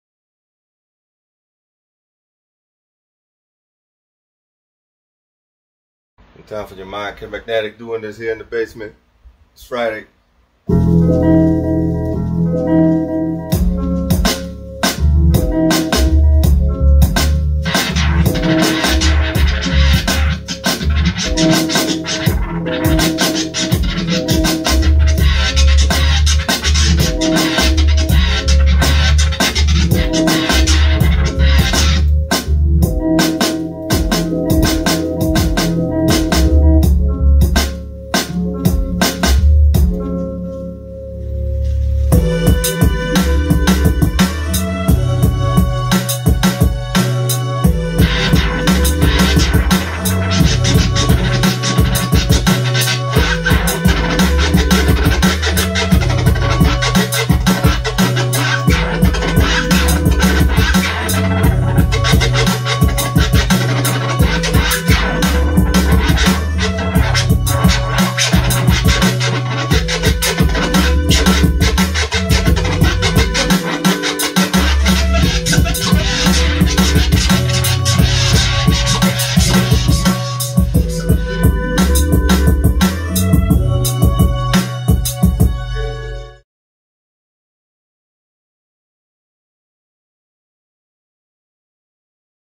K Magnetic Aka Kumachen MPC Mix
Este es K Magnetic Aka Kumachen, Fundador del Boricua BoomBap y Fundador de FunkAdobo, siganme en Facebook @Funkadobo.